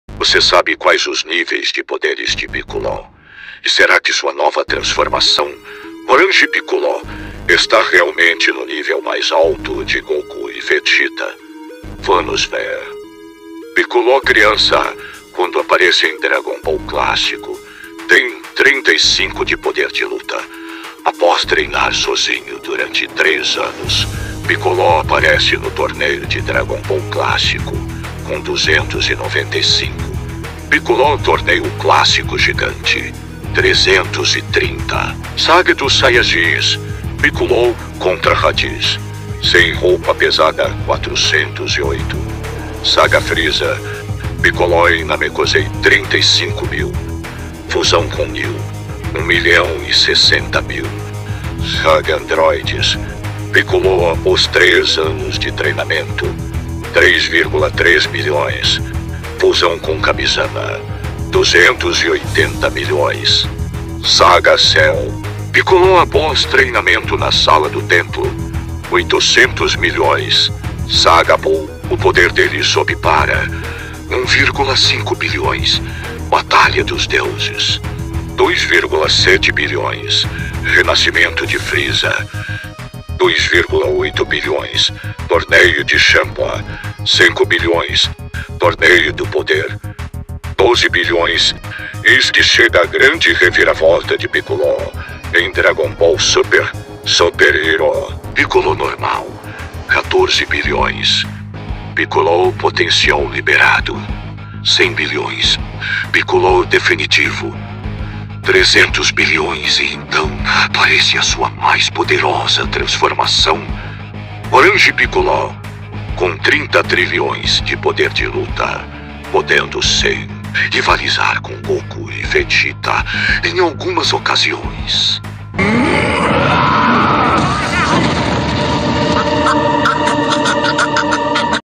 NÍVEL DE PODER DE ORANGE PICCOLO
Você sabe os níveis de poderes de piccolo? e de sua nova transformação o orange piccolo? confira neste vídeo.